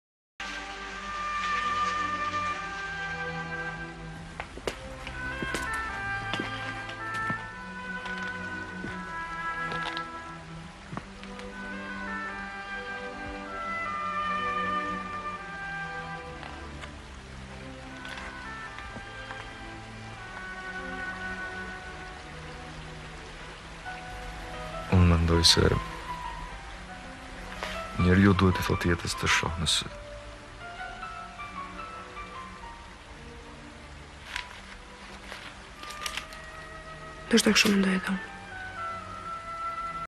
Pozicionim jetësor
Filmat dhe kinematografia përgjithësisht, përcjellin mesazhe të vyera për jetën, mjaft të kesh sy e të dish t'i marrësh këto mesazhe.
I tillë është edhe mesazhi që përcjell pasazhi në fjalë, që jetën duhet t'a shohim në sy.